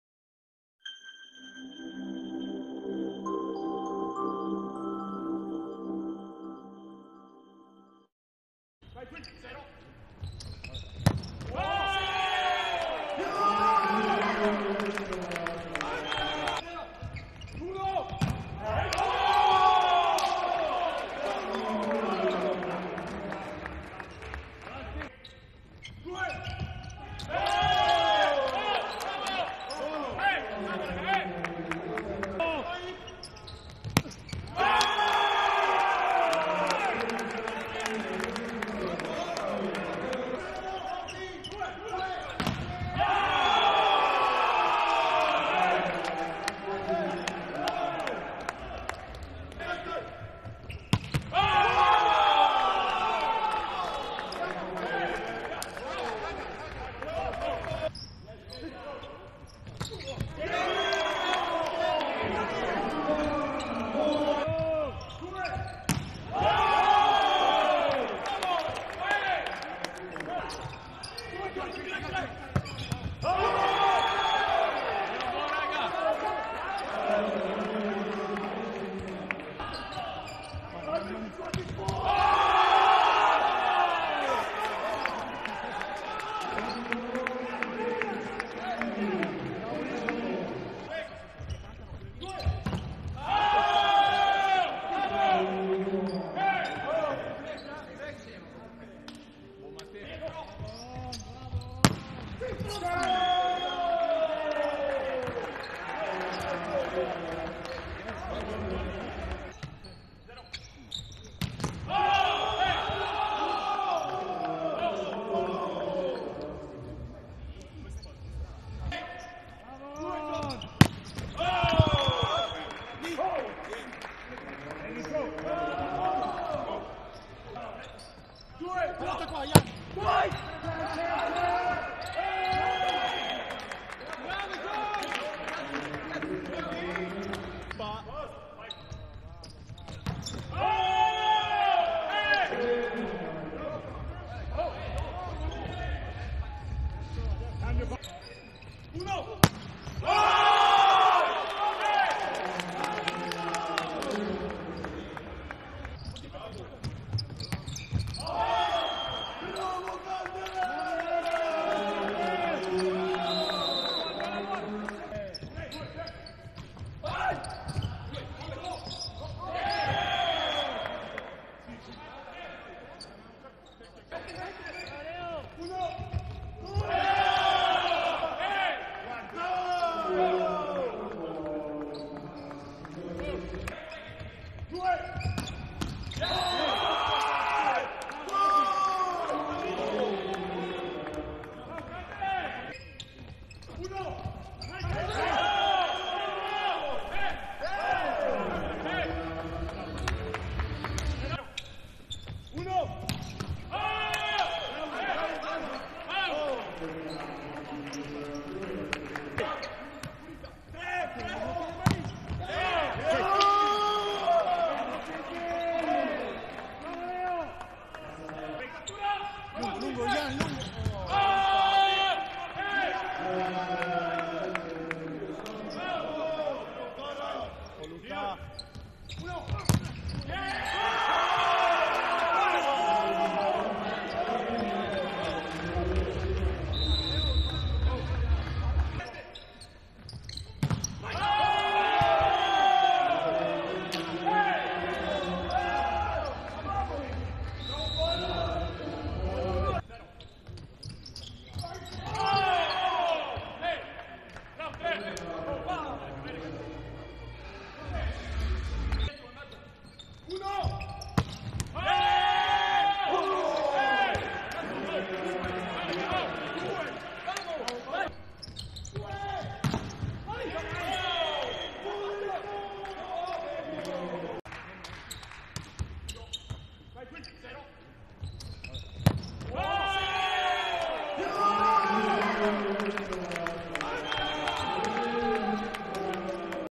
Superlega, highlights Milano - Piacenza
📹 l HIGHLIGHTS
Rivivi gli highlights della sfida tra Allianz Powervolley Milano e Gas Sales Bluenergy Piacenza.
Guarda il video sulla nostra pagina facebook o sul nostro canale youtube.